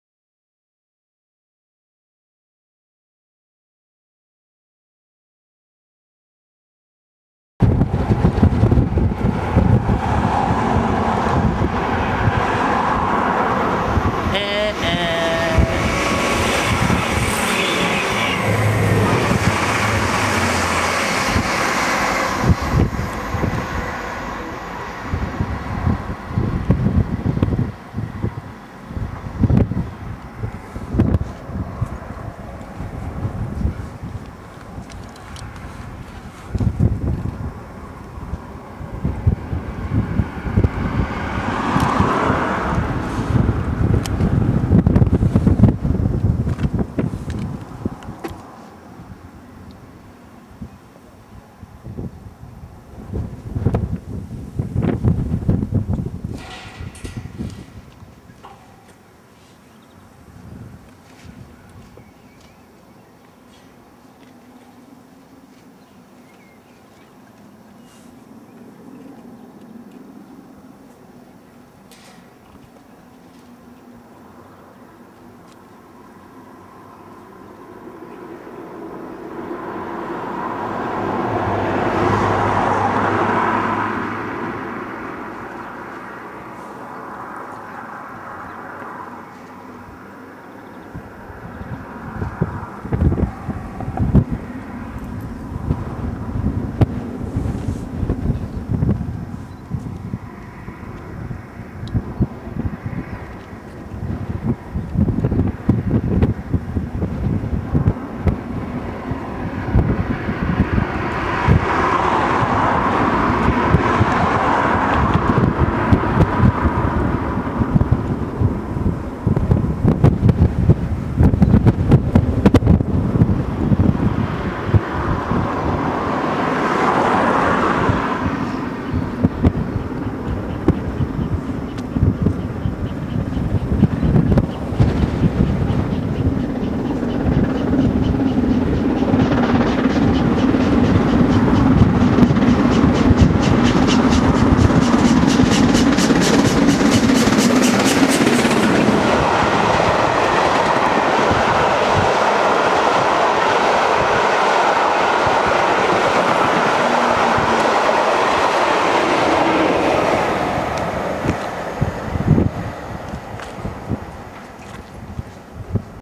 46233 DUCHESS OF SUTHERLAND ON THE YORKSHIRE CORONATION
46233 DUCHESS OF SUTHERLAND ON THE YORKSHIRE CORONATION. LEICESTER TO SCARBOROUGH ON THE 4/4/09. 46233 DUCHESS OF SUTHERLAND IS PASSING ULLESKELF TRAVELLING FROM CHURCH FENTON STATION IN THE DISTANCE ON ITS WAY TO SCARBOROUGH